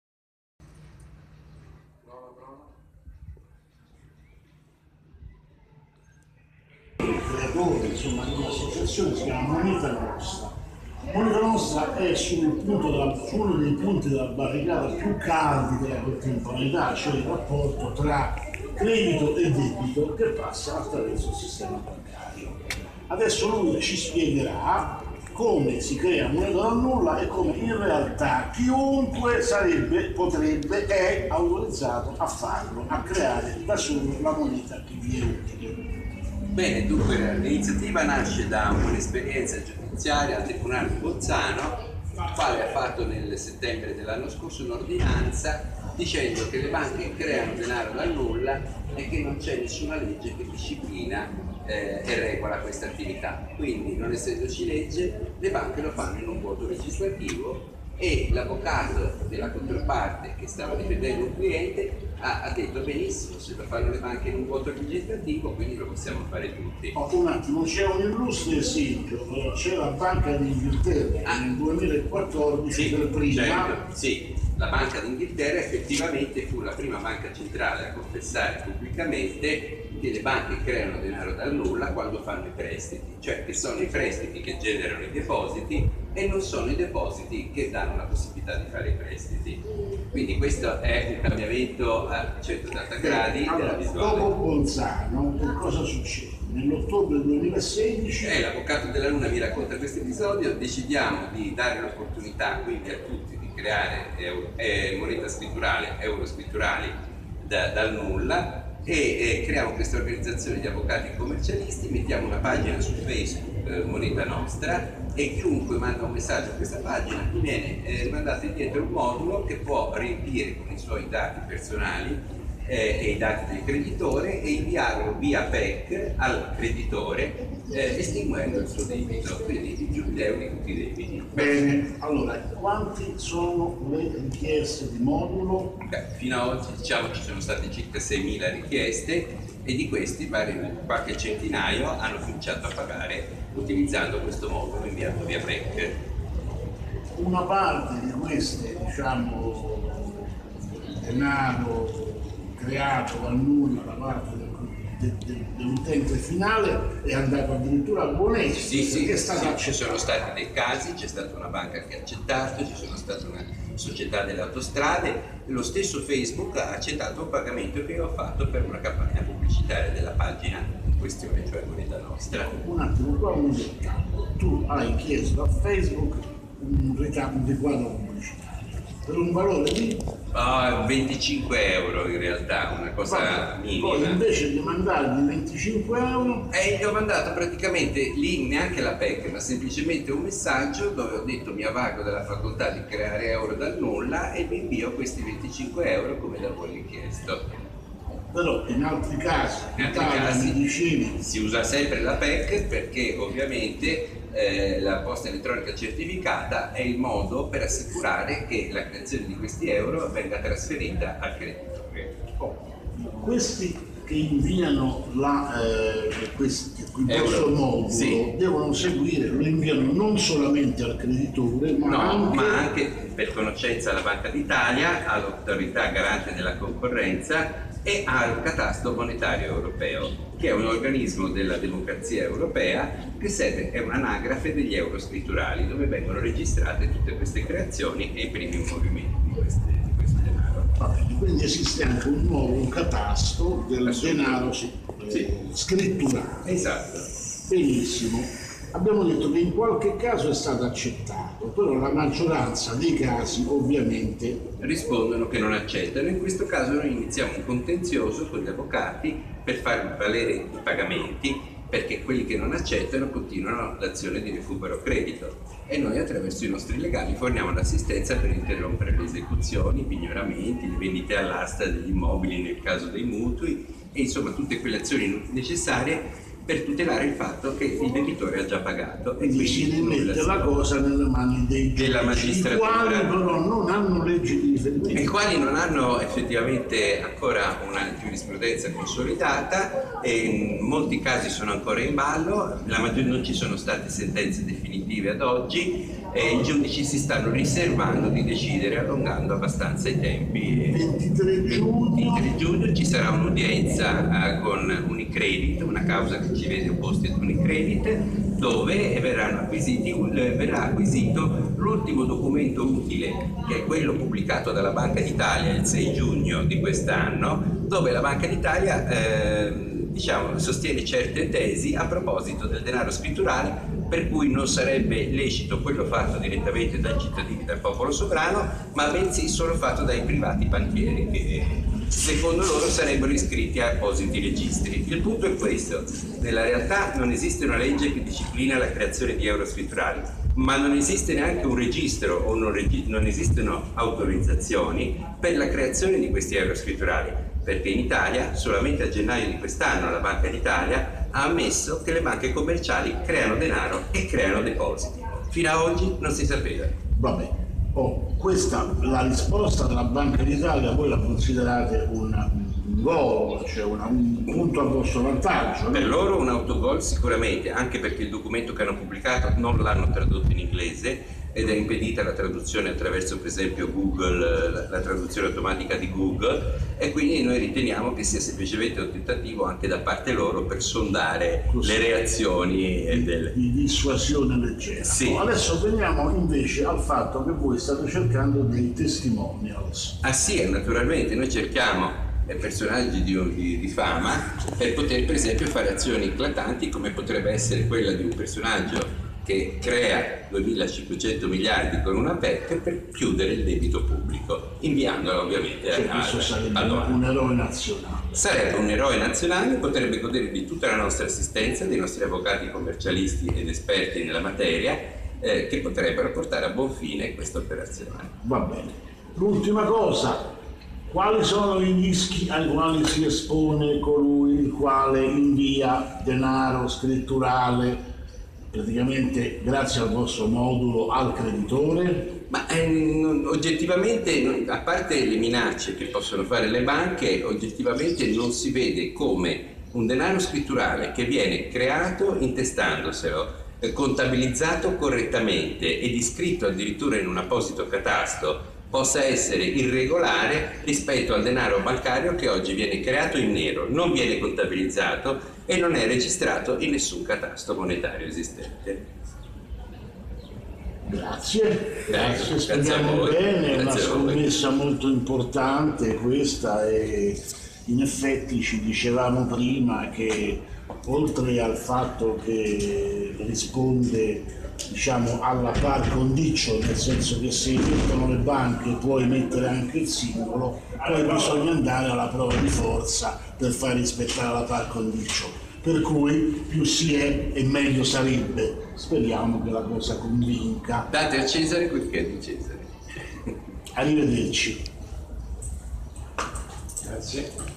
Moneta Nostra: Glauco Benigni intervista Marco Saba
Glauco Benigni intervista Marco Saba su Moneta Nostra in occasione del PANDORA-DAY a Roma il 10 giugno 2017 - Ripresa amatoriale di: Nadia La Banca (M5s)